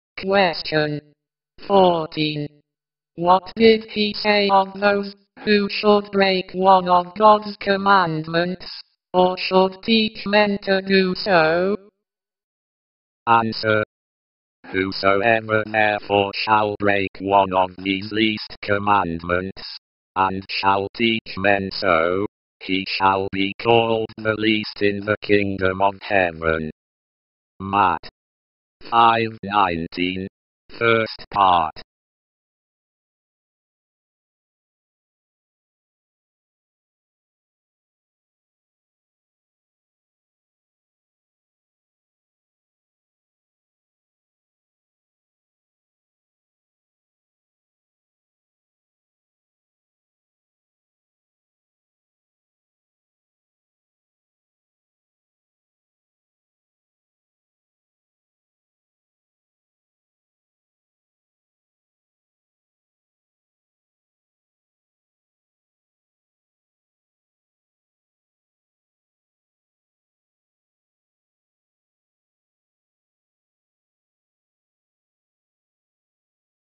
14. What did He say of those who should break one of God's commandments, or should teach men to do s
14. What did He say of those who should break one of God's commandments, or should teach men to do so?
Answer:
"Whosoever therefore shall break one of these least commandments, and shall teach men so, he shall becalled the least in the kingdom of heaven." Matt. 5:19, first part.